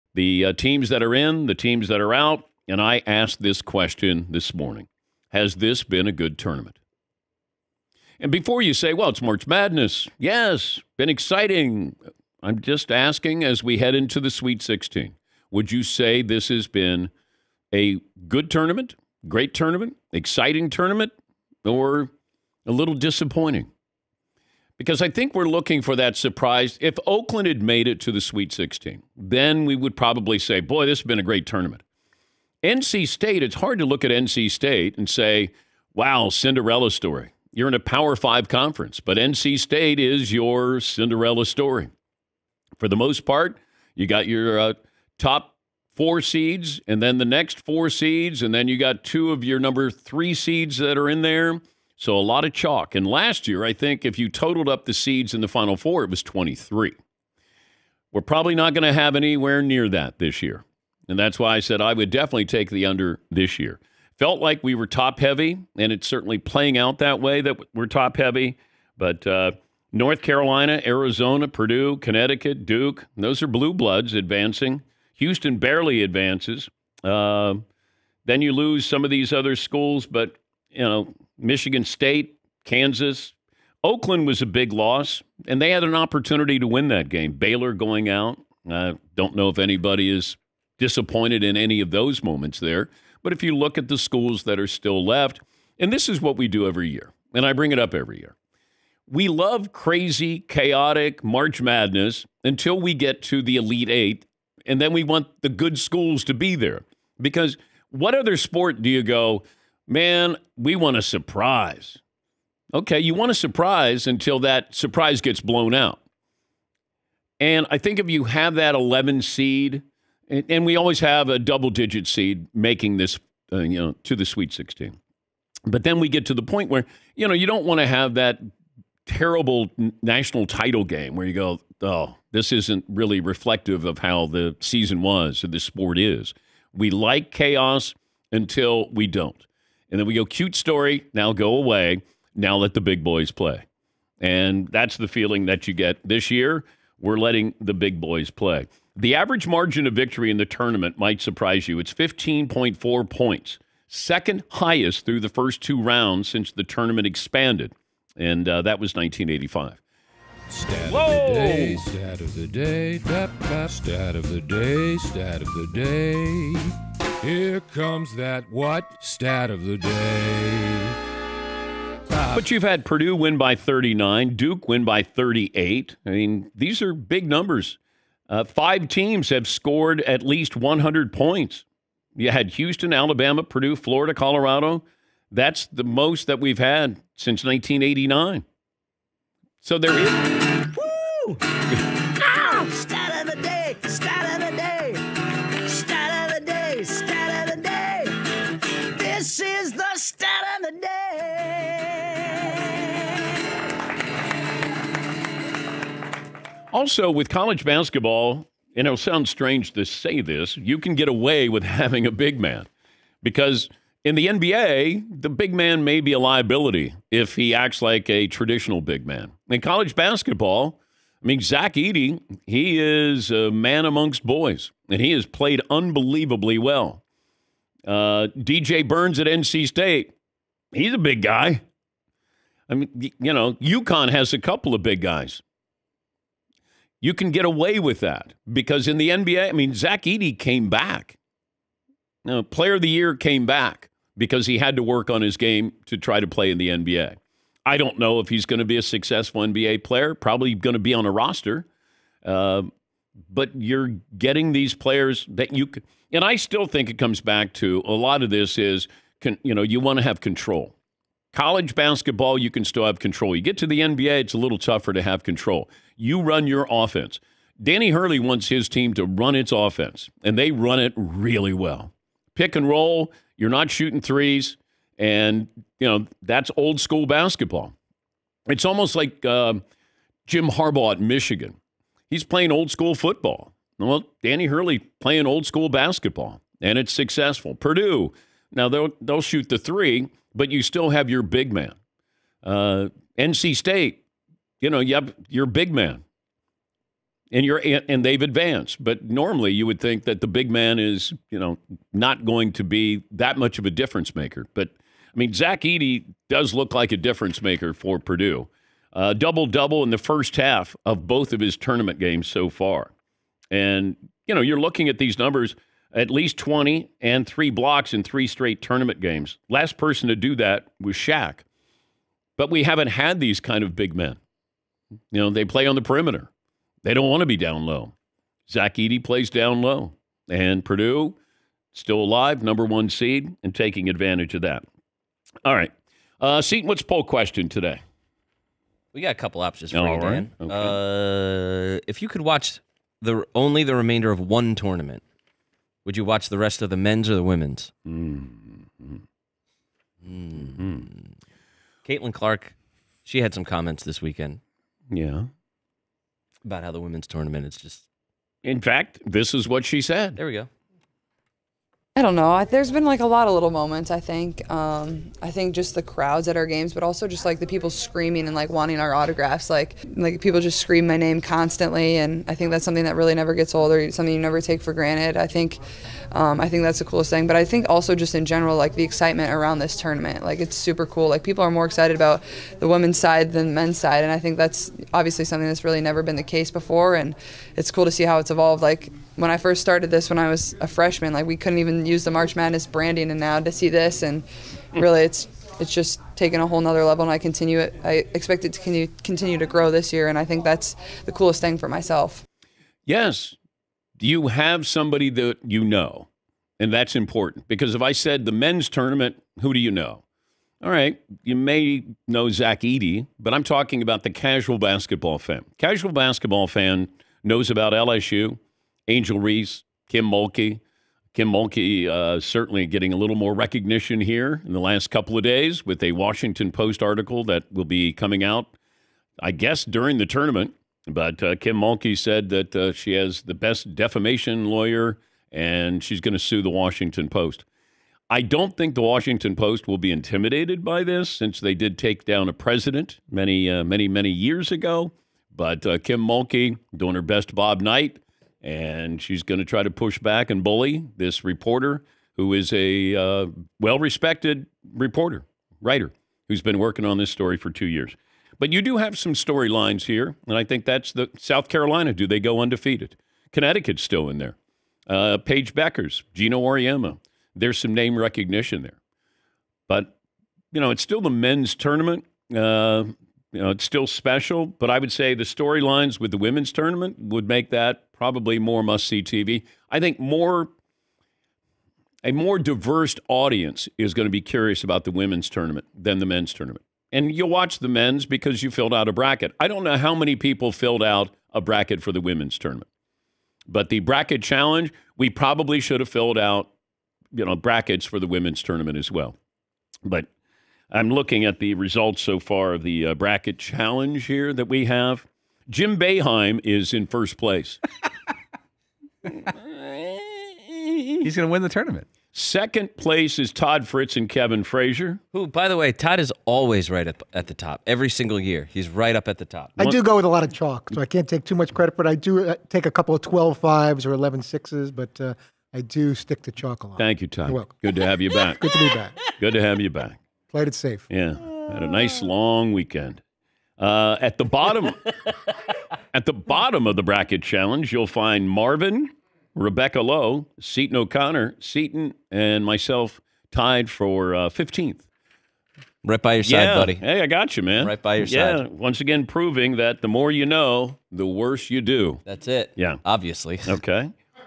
Has This Been A Good NCAA Tournament This Year? | 3/25/24
Dan Patrick recaps opening weekend to the NCAA Tournament and asks whether or not the tournament has been good this year